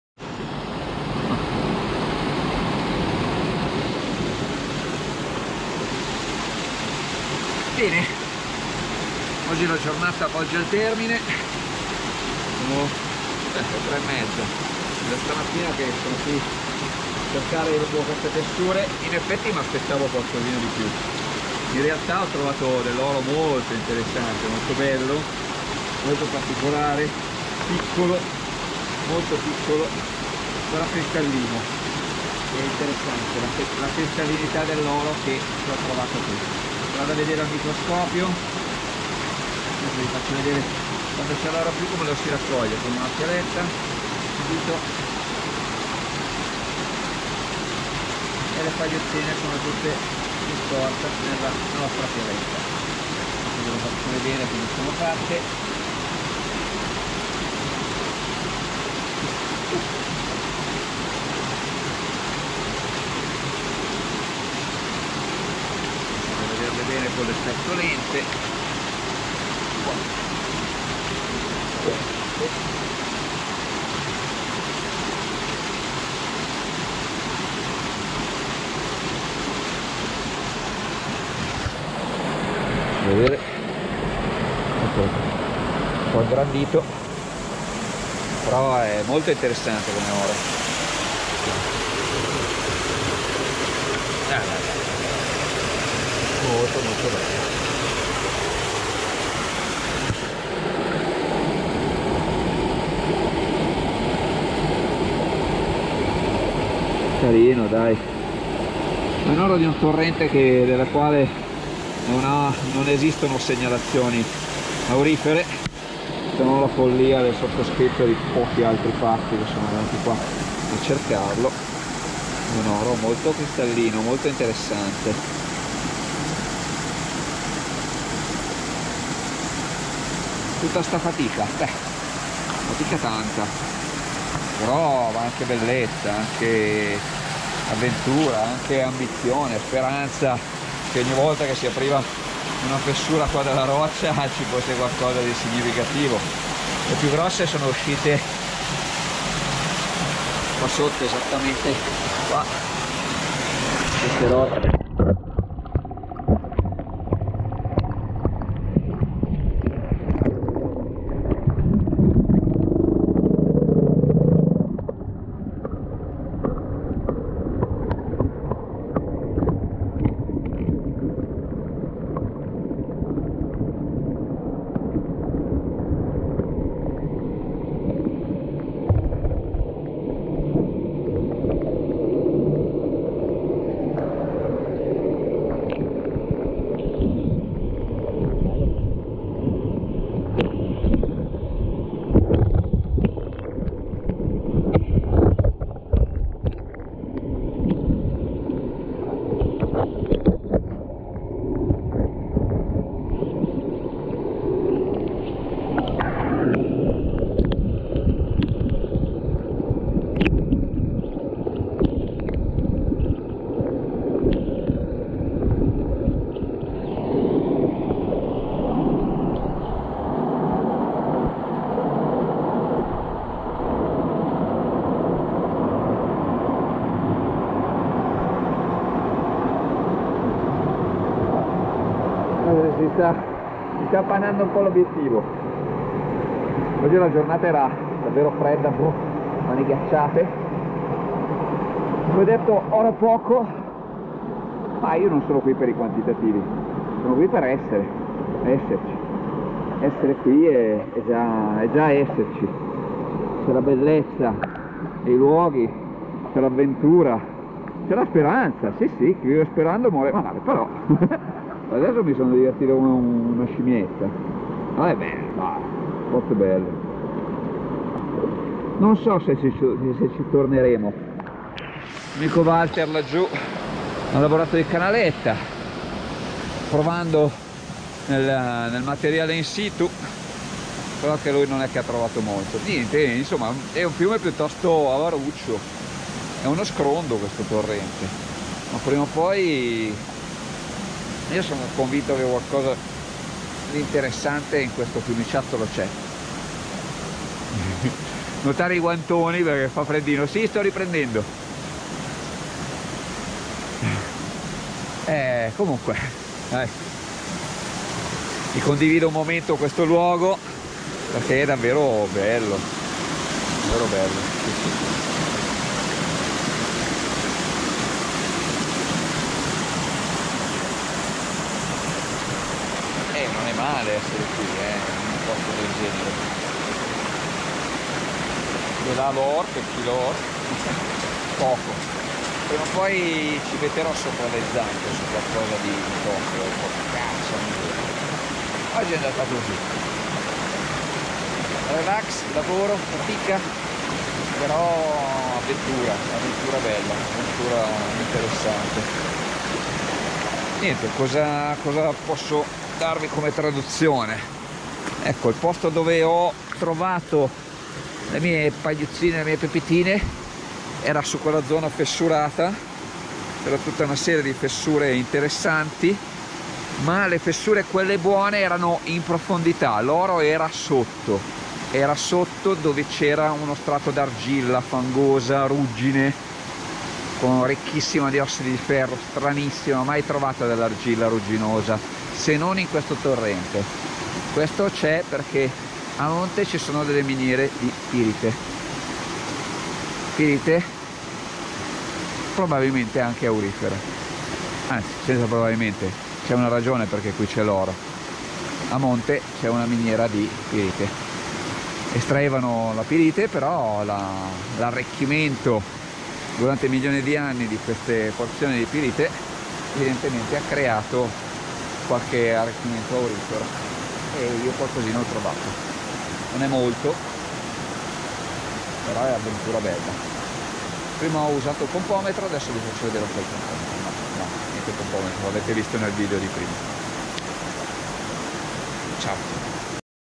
oro della valle Antrona
Cercando oro in Valle Antrona, il finale di giornata, un passaggio sott'acqua ad ascoltare il suono del fiume e qualche parola su questo hobby.